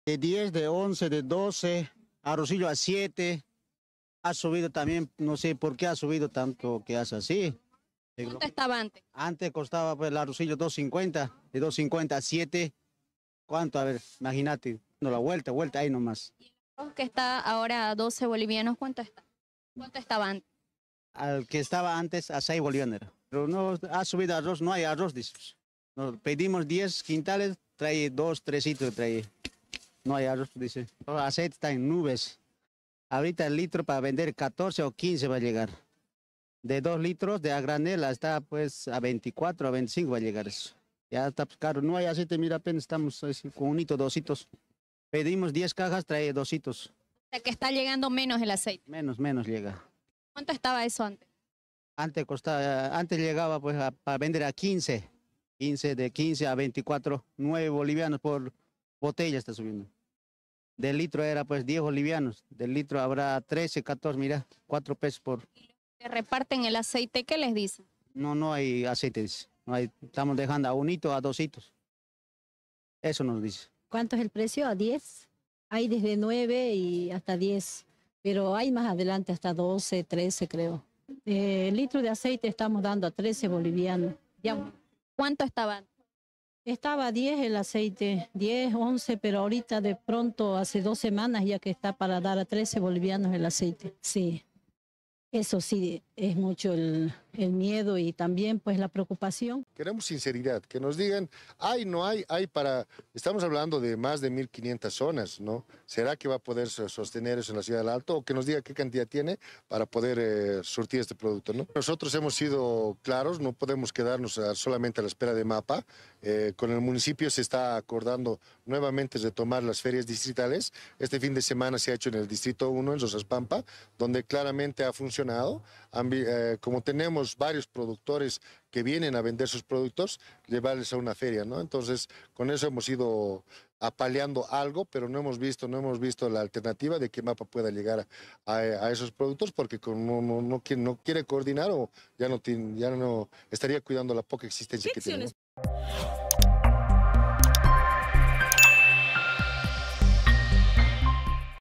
Sube el precio del arroz y aceite
Vecinos reportan una drástica alza en los precios del arroz y el aceite, que dificulta el abastecimiento en la ciudad. Productos como el arroz pasaron de Bs 2.50 a Bs 7, mientras que el litro de aceite subió de Bs 10 a Bs 13. Las autoridades evalúan soluciones en ferias distritales para mitigar la escasez y el impacto económico.